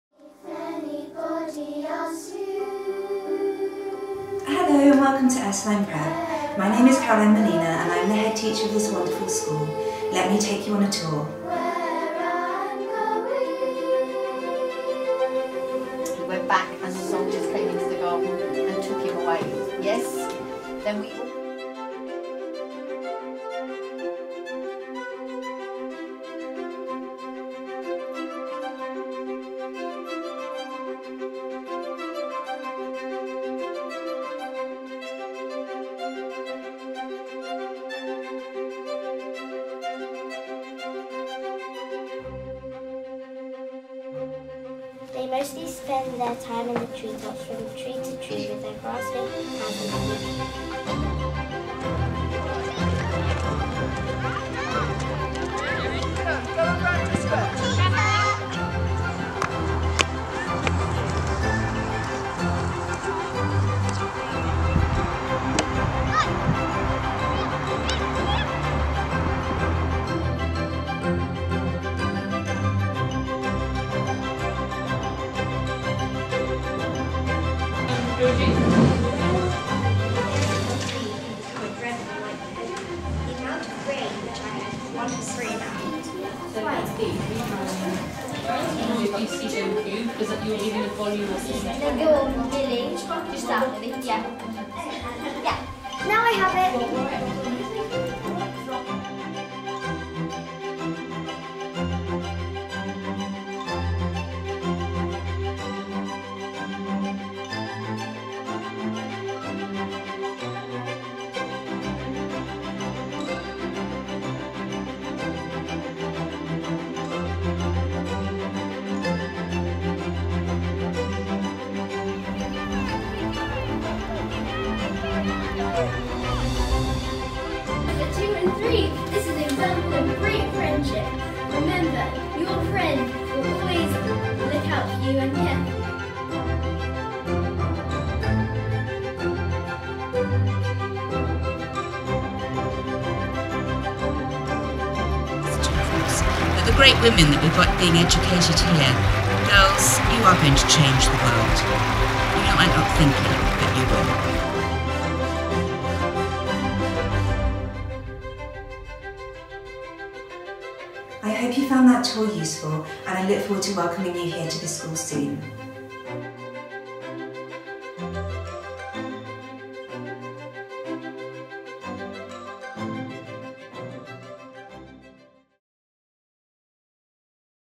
Ursuline Preparatory School - Prep Department
A taster of the Ursuline Prep department and the wonderful experiences we can offer your girls. @UrsulinePrep